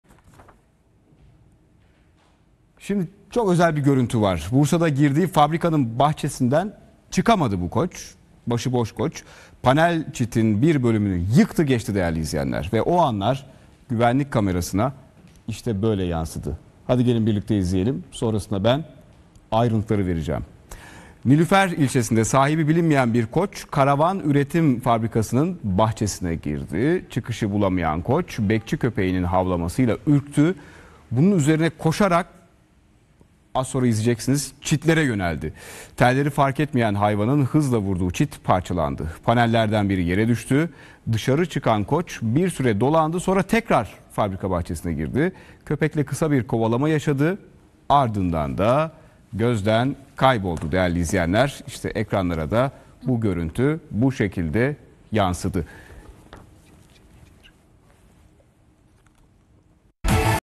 Başıboş KOÇ fabrikanın çitlerini kırdı yetmedi köpeği kovaladı
Bursa'da sahibi bilinmeyen bir koç, karavan üretim fabrikasının bahçesine girdi.
Çıkışı bulamayan koç, bekçi köpeğinin havlamasıyla ürktü.
Bunu üzerine koşarak çitlere yöneldi.
Telleri fark etmeyen hayvanın hızla vurduğu çit parçalandı.
Panellerden biri yere düştü.
Dışarı çıkan koç, bir süre dolandıktan sonra tekrar fabrika bahçesine girdi.
Köpekle kısa kovalamaca yaşayan koç, ardından gözden kayboldu.